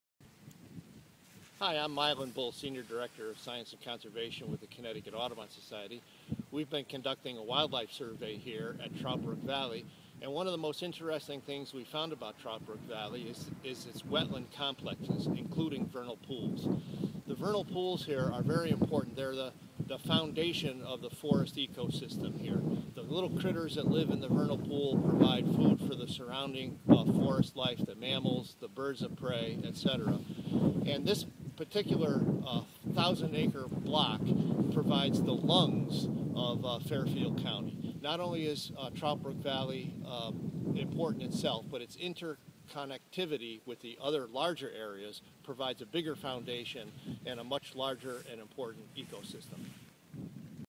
Why Vernal Pools Matter
Milan Bull, CT Audubon Senior Director of Science and Conservation discusses why vernal pools in Trout Brook Valley are so important